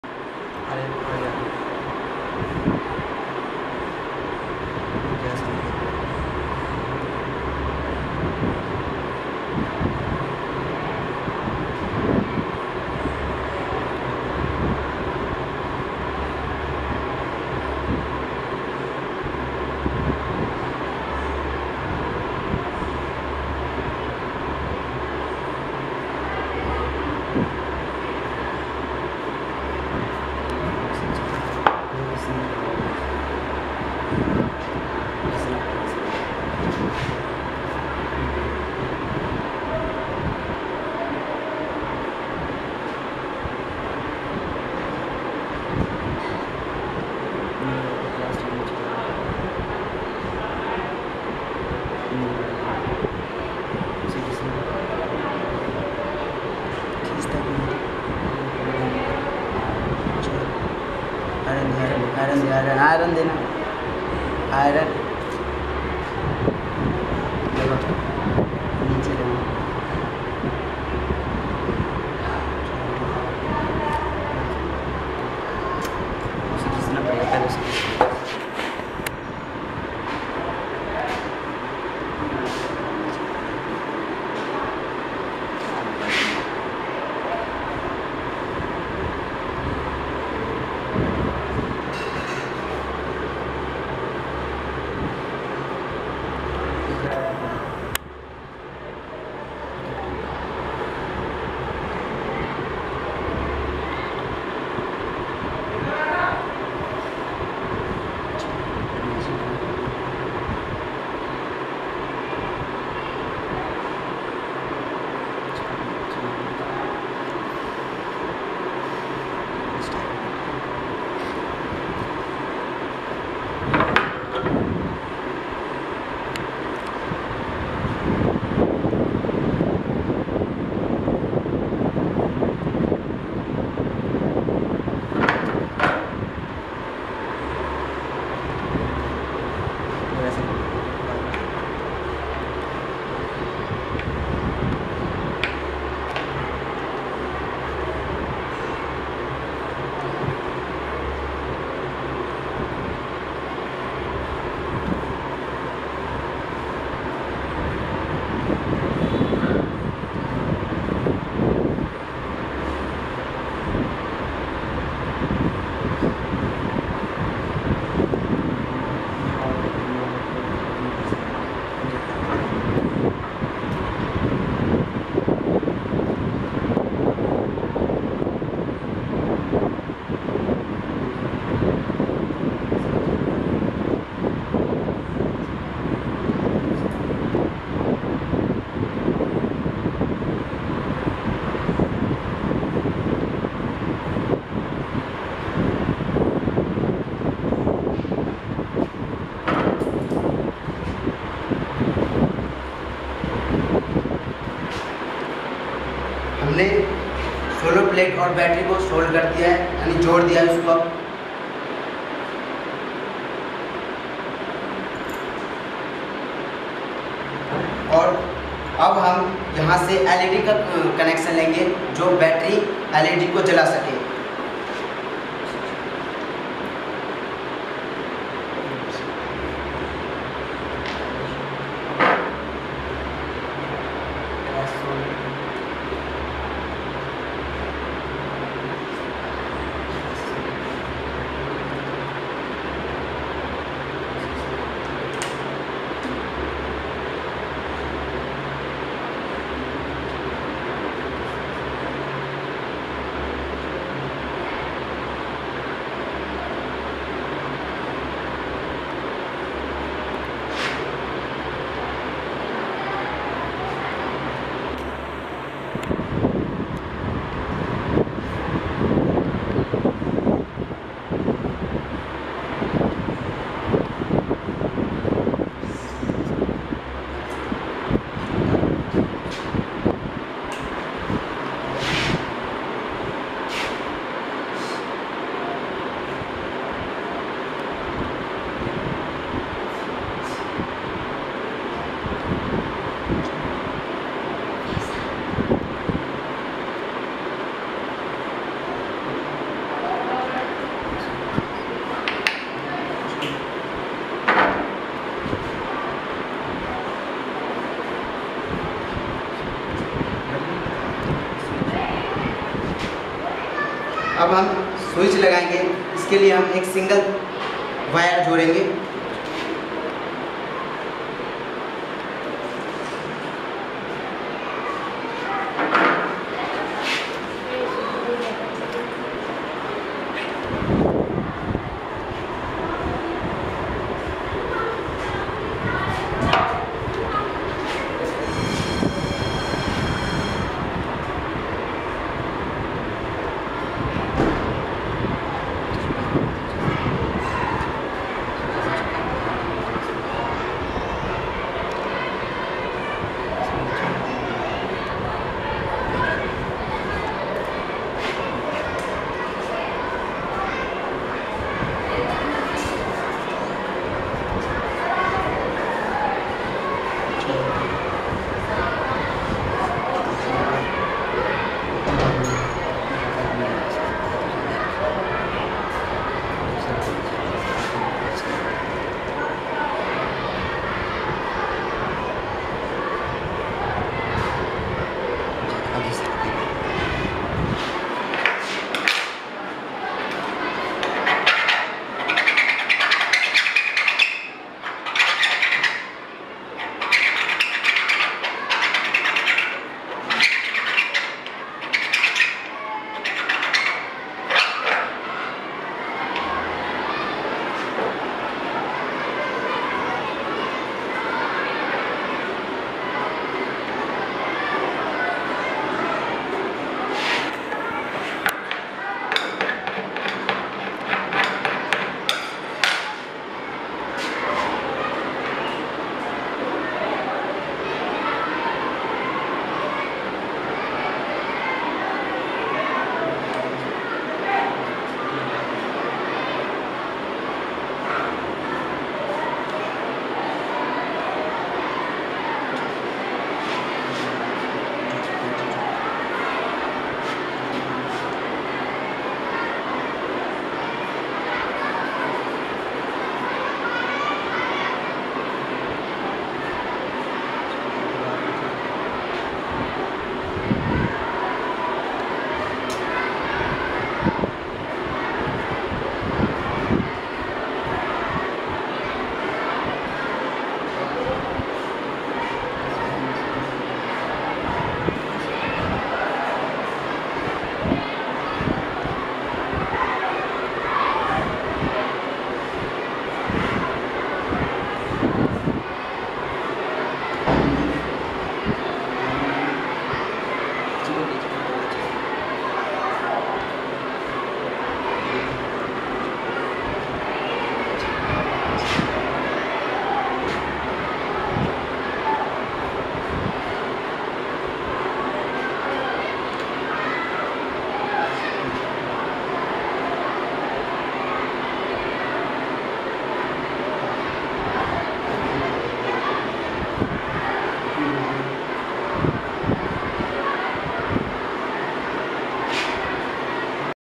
Solar powered School Bag making process by Literacy india Children
Literacy India Projeect Sampurna Shiksha ( Holistic Education)